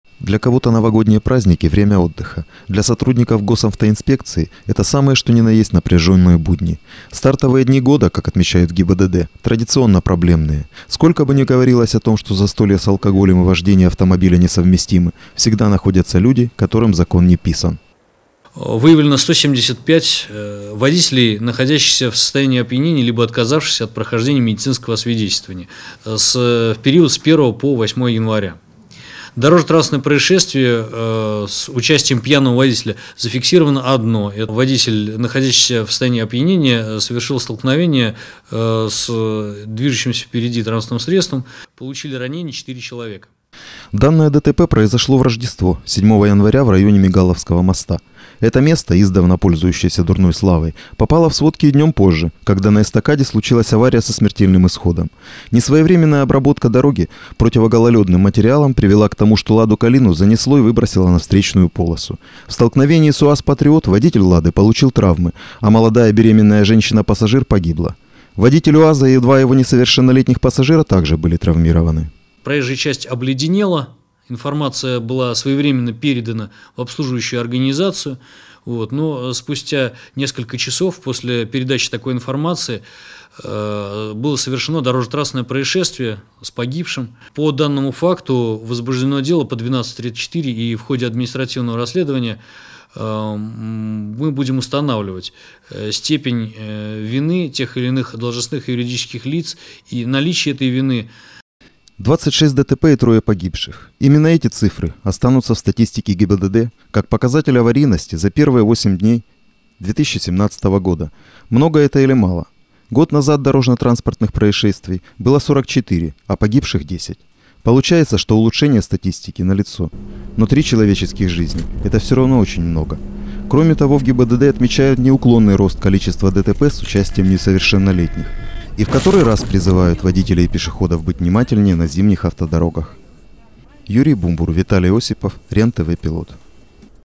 АВАРИИ НА ТВЕРСКИХ ДОРОГАХ
Для сотрудников ГИБДД первые дни года – это не только и не столько праздник. Традиционно начало января один из самых неблагоприятных периодов с точки зрения ДТП. Старт 2017 года отчасти подтвердил это. С одной стороны дорожно-транспортных происшествий было зафиксировано меньше, чем годом ранее. С другой – без трагических последствий все-таки не обошлось.